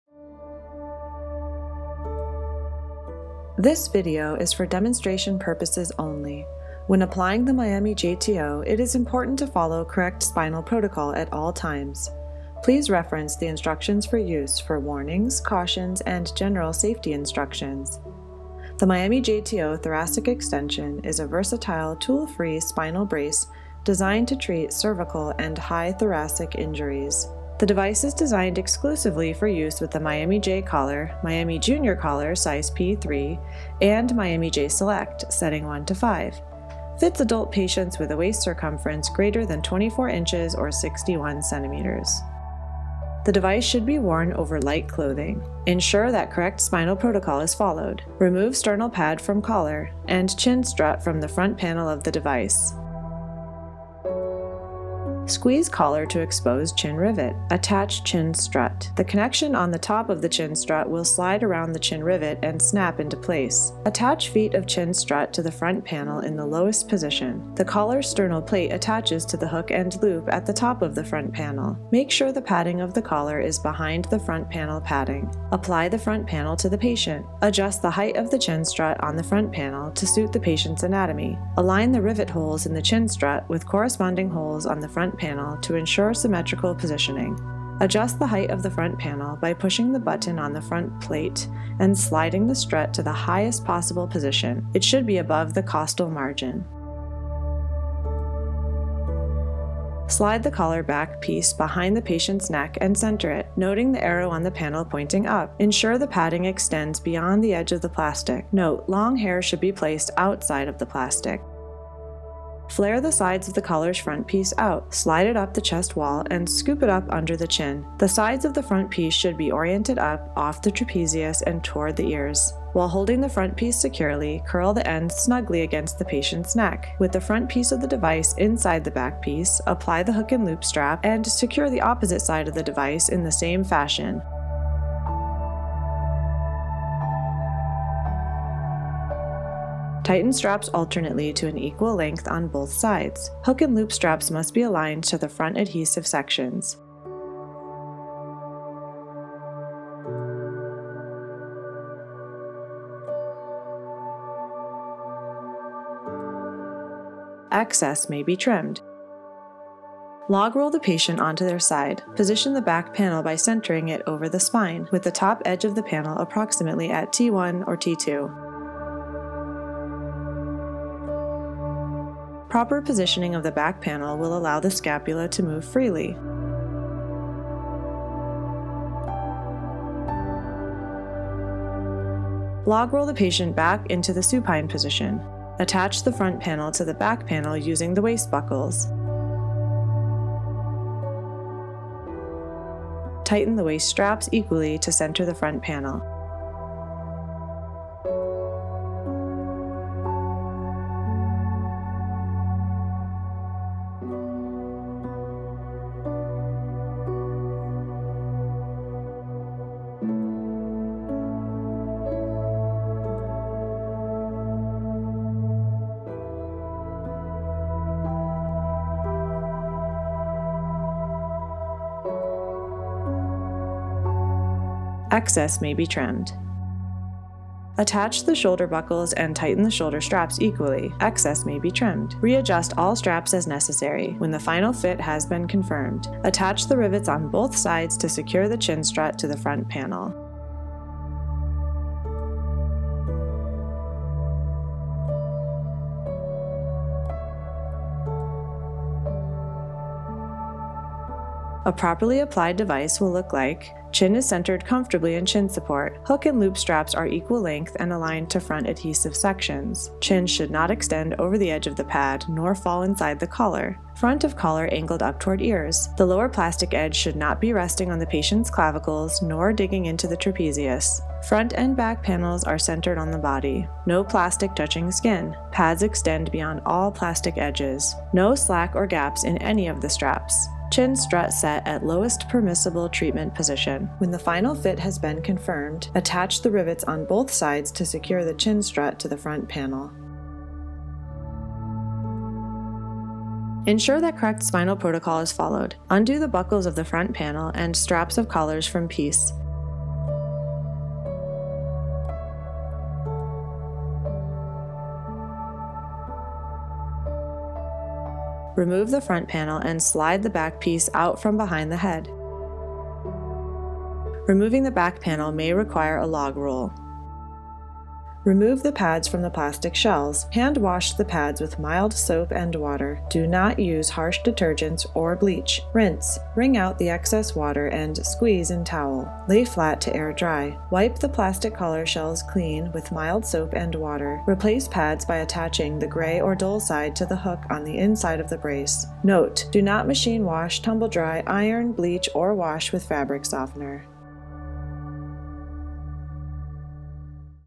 Miami JTO Thoracic Extension Brace (Clinician Fitting Tutorial)
Miami JTO® Thoracic Extension
Miami JTO Thoracic Extension is designed in conjunction with leading spine surgeons, orthotists and nurses. The orthosis is used exclusively with all Miami J® collars when even greater stability at the cervical junctions is indicated.
Miami JTO Thoracic Extension is a versatile, tool-free device with a patented chin strut, providing more control than a collar alone can provide, designed to treat cervical and high-thoracic injuries.